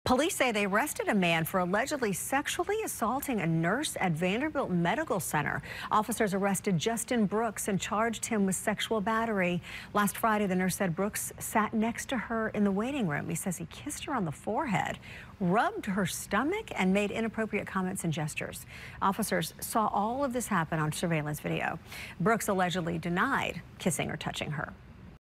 Nurse sexually assaulted
Vanderbil University Police officers arrested a man after a nurse accused him of touching her and saying inappropriate things.